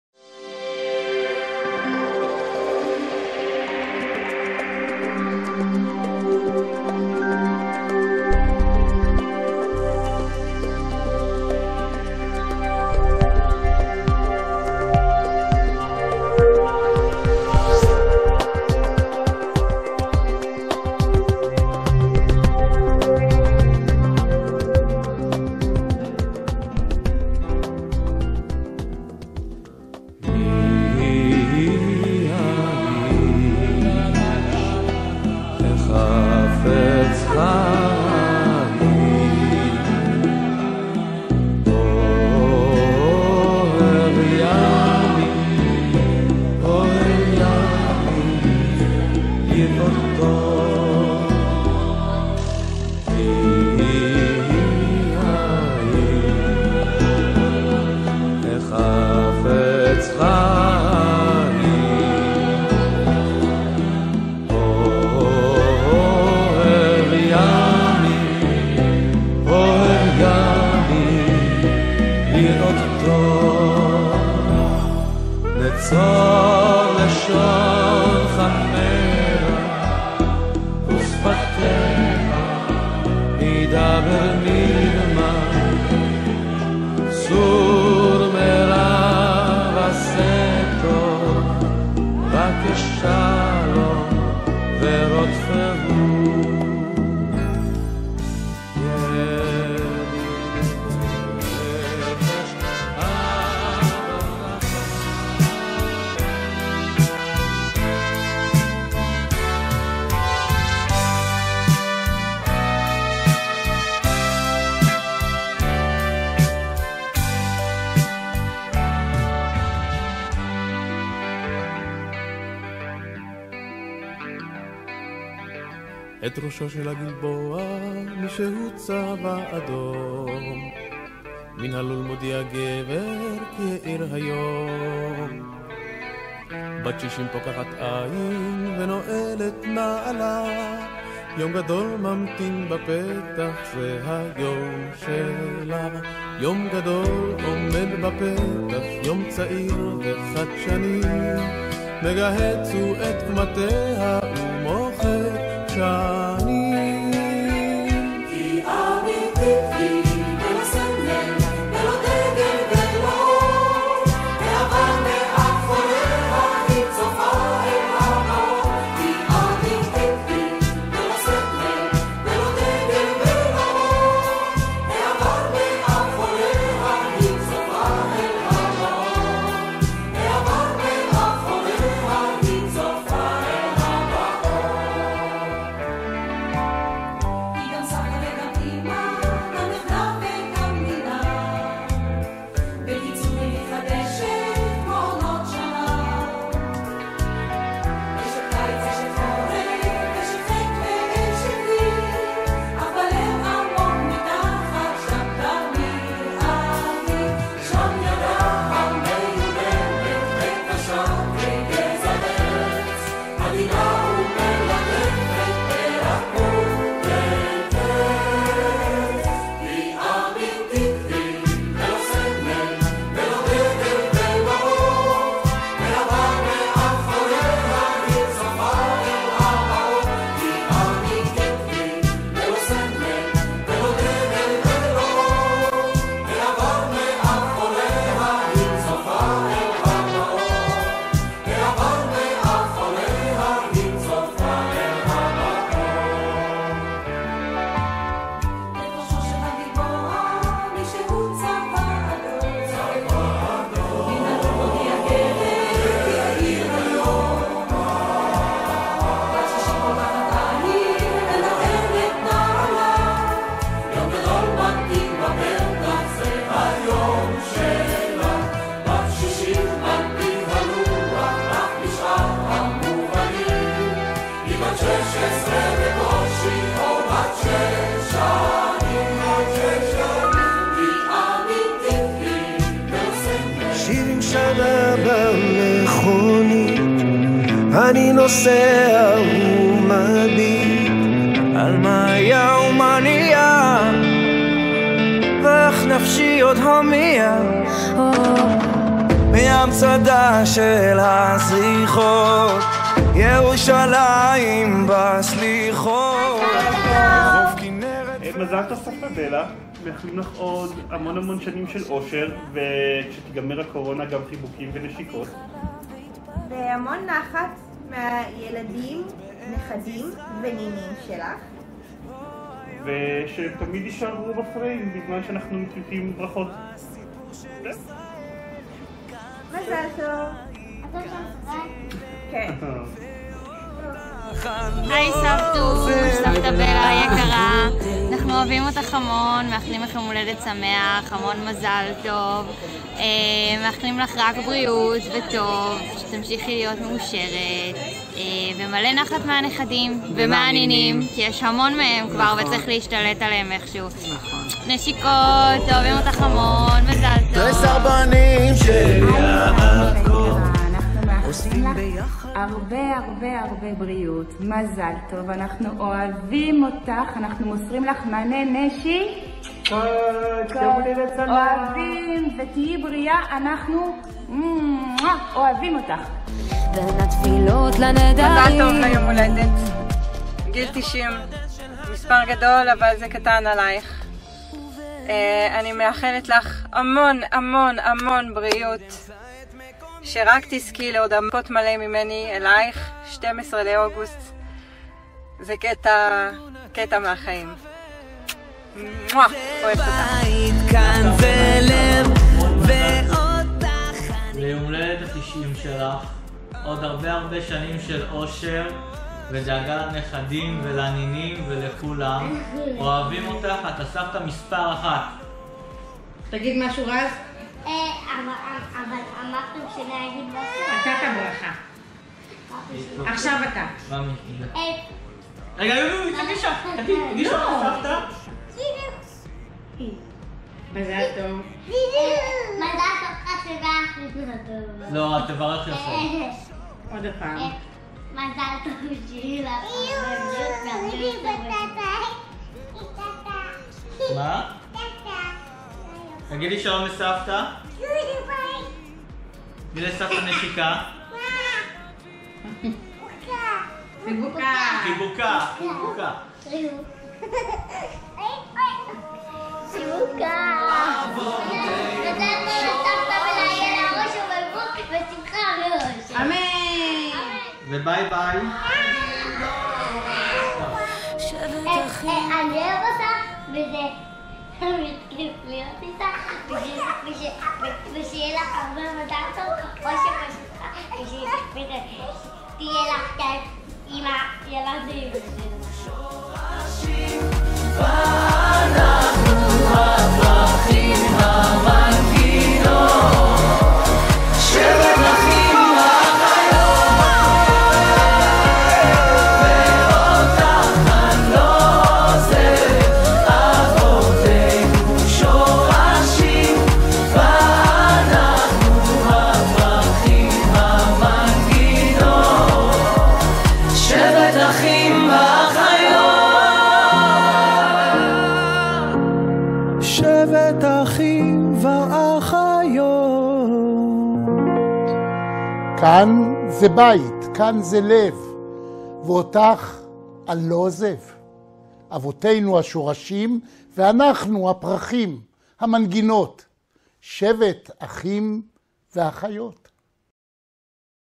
BELA 90
סירטון שהוכן ליום ההולדת 90 של בלה צבינר